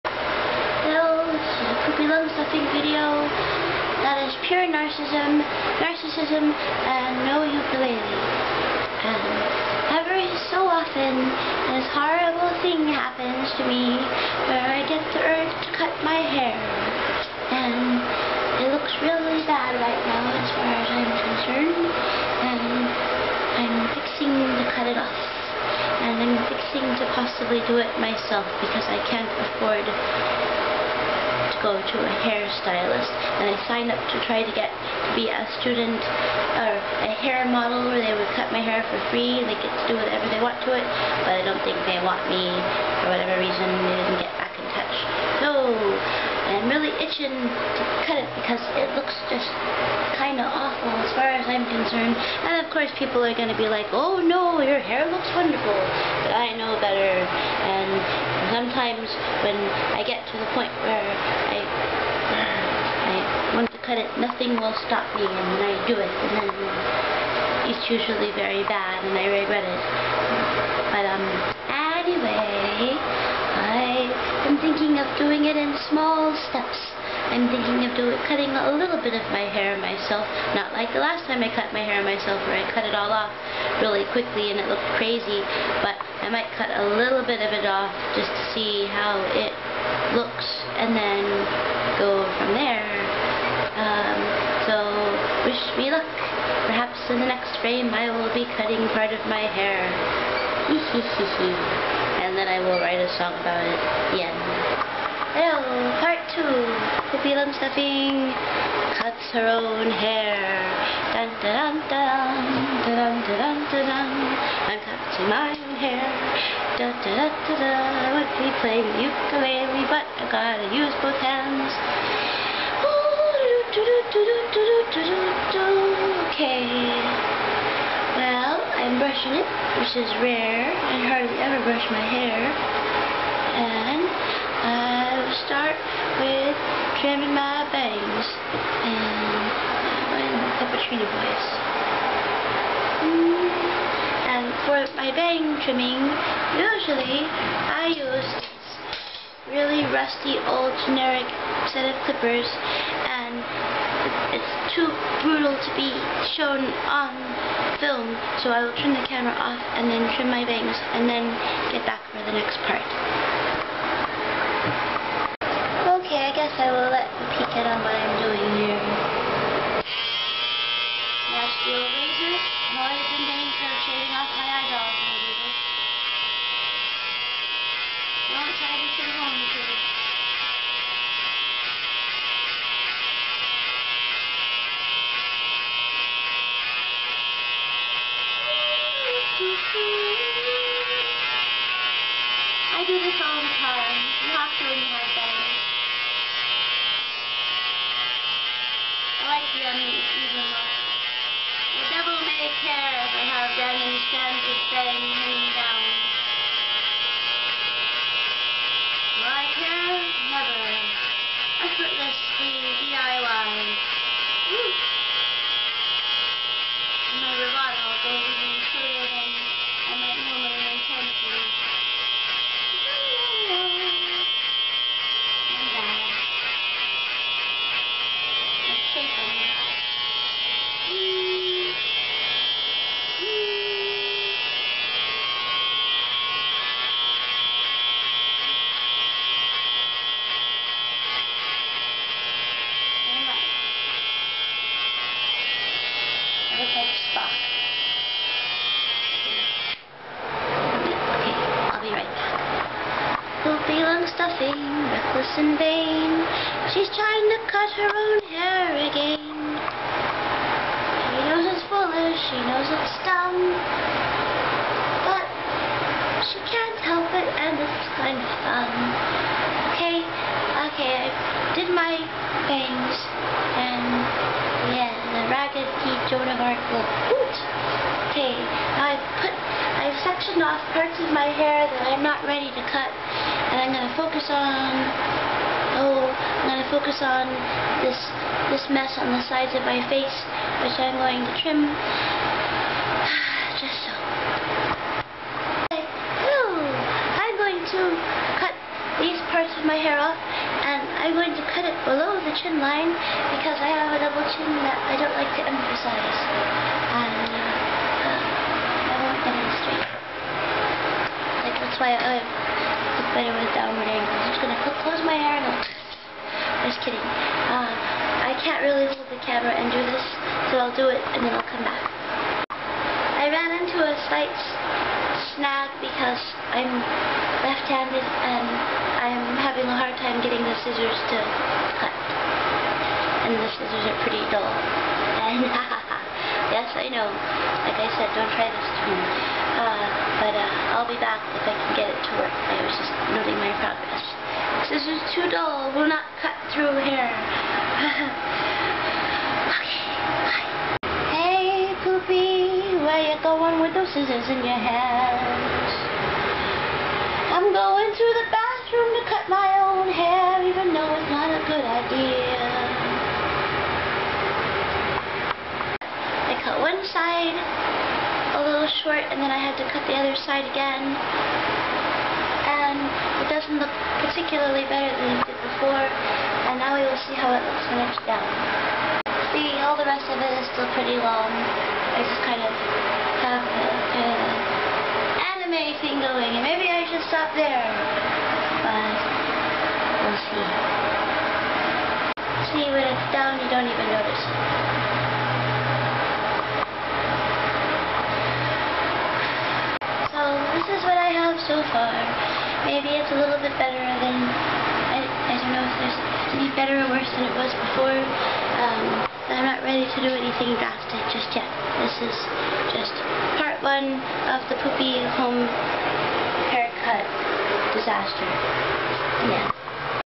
Poopy Lungstuffing's home haircut horrors (part 1)
Occasionally, for whatever reason, I am overcome with the intense urge to do something with my hair. I am afraid of going to a cheapo salon...and I can't afford to go to an expensive salon...and it doesn't really matter anyway because I wind up frozen like a deer in the headlights and lose the ability to communicate with the stylist and always walk out with the same kind of bob no matter what.
Anyway...I have decided to take matters into my own hands...Right now, I am in phase 1